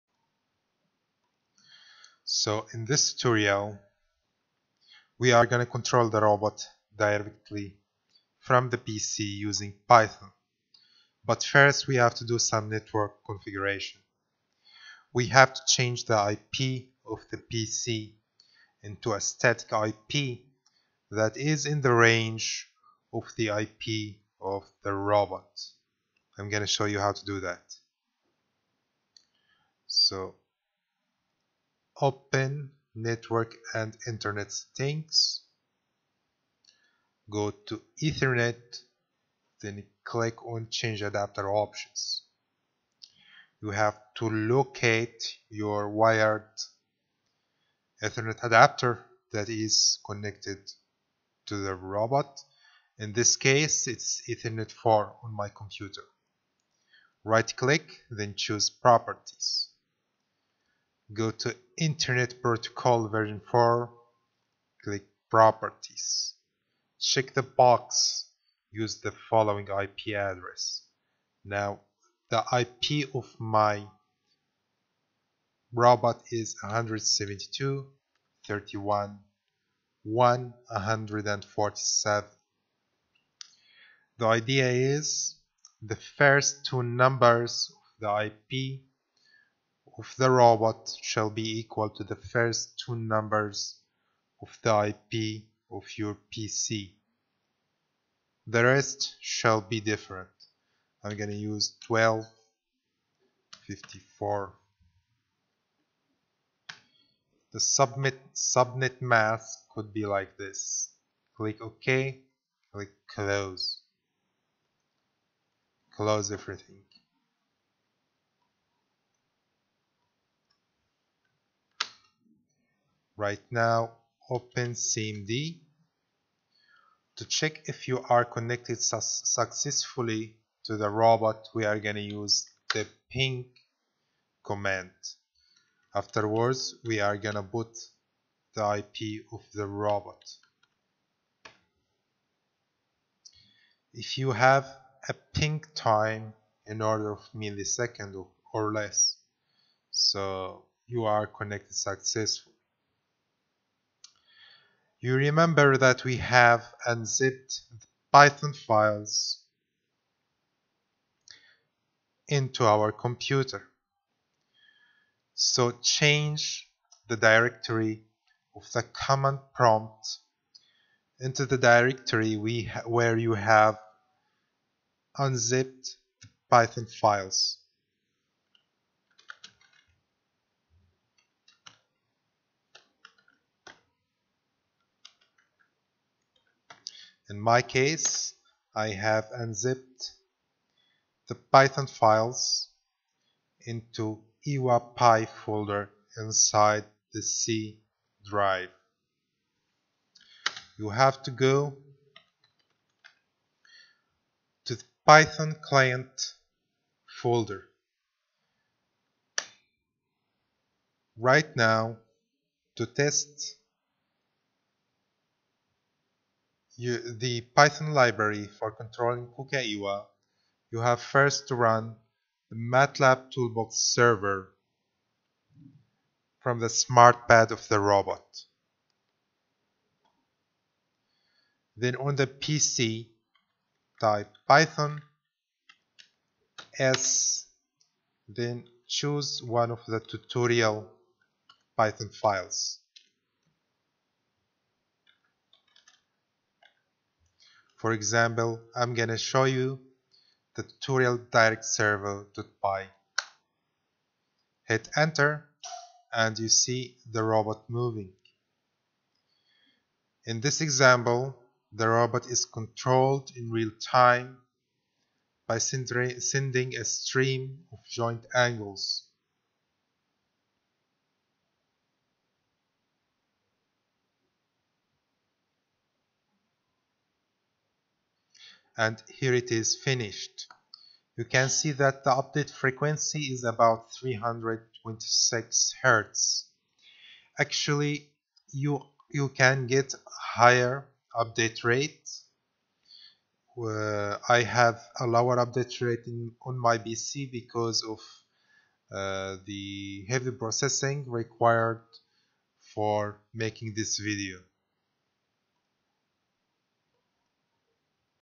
Controlling KUKA iiwa from PC using Python #03
In this tutorial, the robot is moving according to commands in a Python script on the external PC.